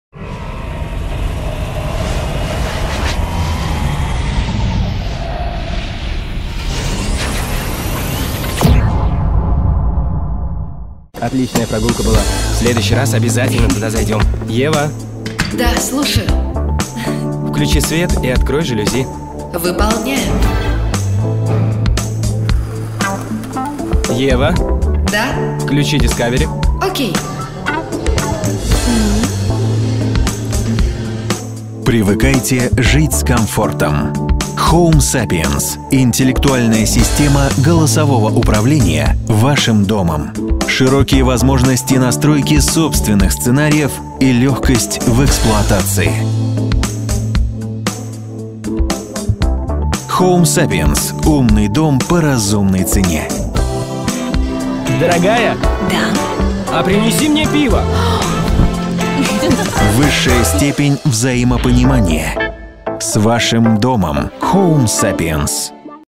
Видео Home Sapiens об умном доме
Комбинированный ролик (съемка+компьютерная графика) презентует систему управления апартаментами или домом Home Sapiens.